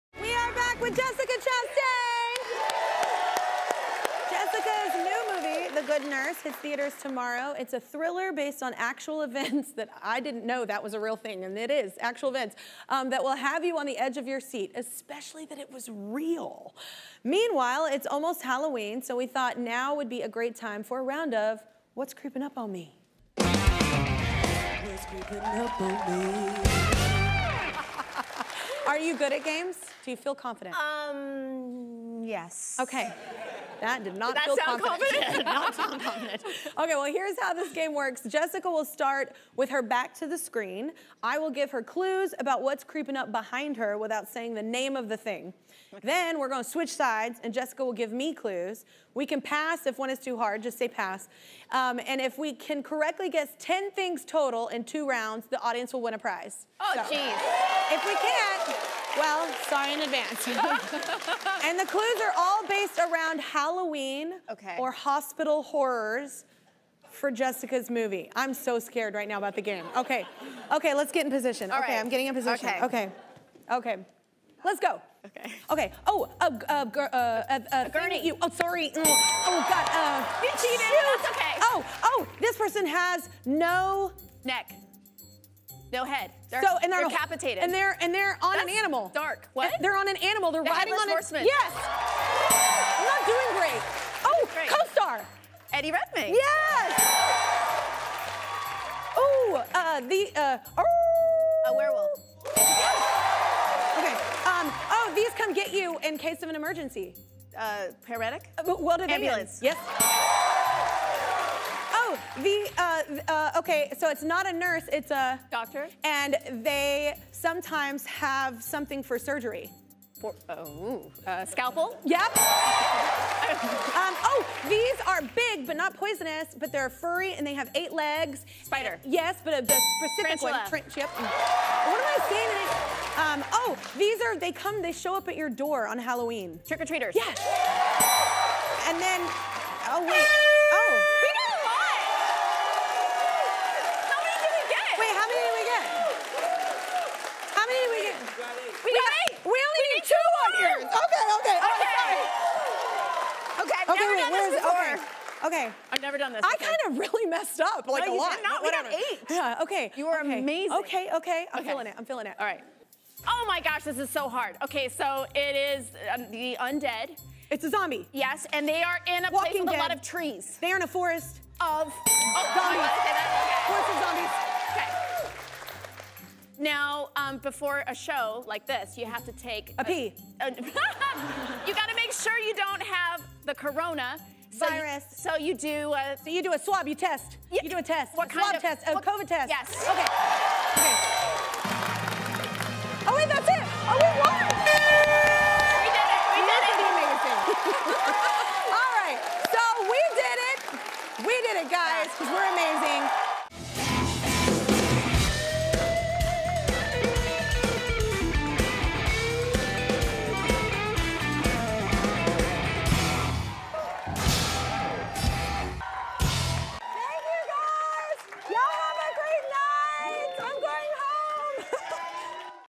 Jessica Chastain & Kelly Clarkson Team Up In 'What's Creepin' Up On Me?' Game
"The Good Nurse" star Jessica Chastain and Kelly are getting ready for Halloween with another round of "What's Creepin' Up On Me?" a game where they guess what spooky thing is on the green screen behind them!